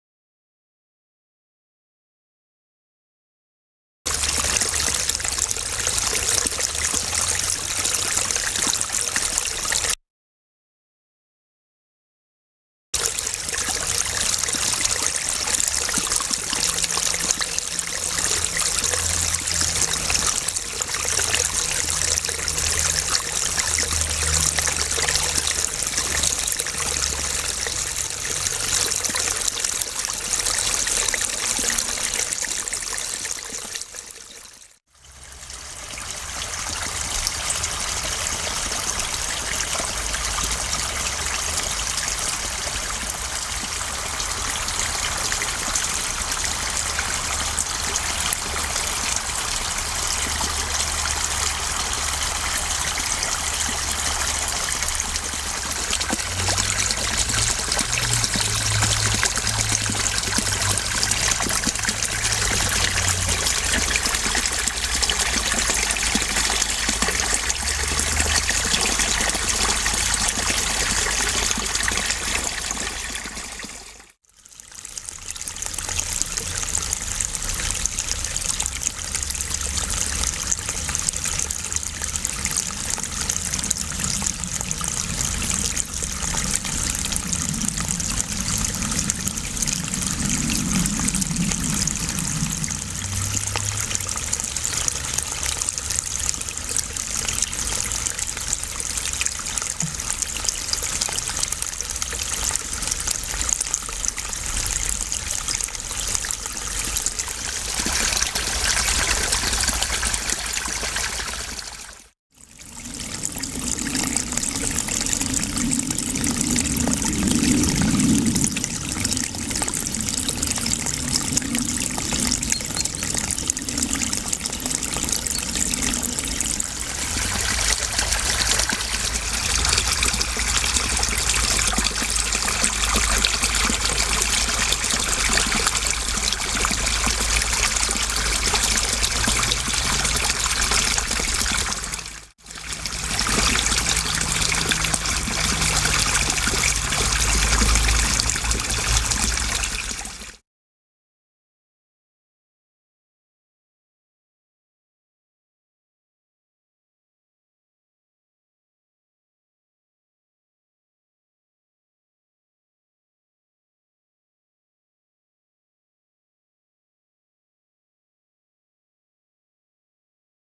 Wasser/ Water - Ein erstes Video mit/ A first movie with: Canon EOS 550D / Rebel T2i
Ein erster Versuch einen Film mit meiner Canon EOS EOS 550D zu machen /
A first try to make a movie with my Canon EOS Rebel T2i

Objektiv: Canon  EF 100 mm 2.8   Macro USM
Software: Pinacle VideoSpin

Weitere Filme folgen hoffentlich bald. / More movies come hopefully soon.